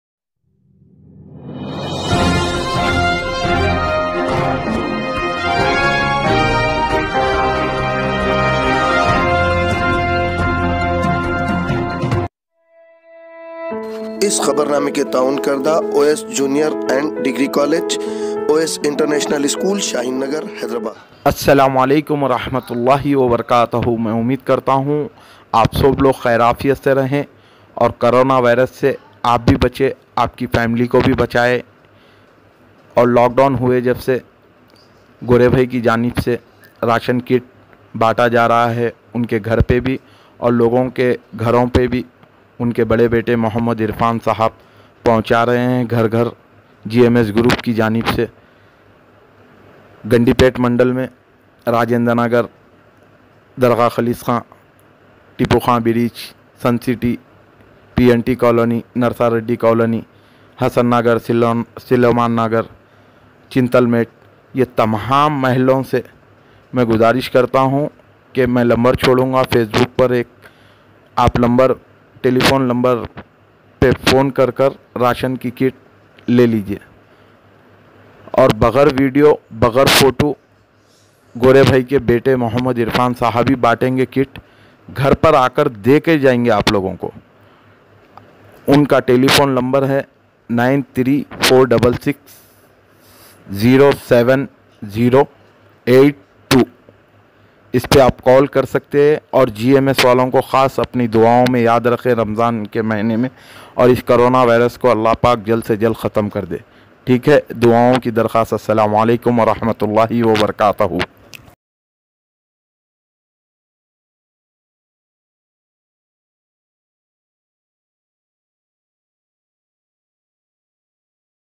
Today afternoon daily Times news Bureau chief S A M M Quadri meet chairman gms group Mr Gore bhai el
Today afternoon daily Times news Bureau chief S A M M Quadri meet chairman gms group Mr Gore bhai elder son Mr Mohammed Irfan and Mohammed Afroz is starting Roshan kit distribution poor and needy people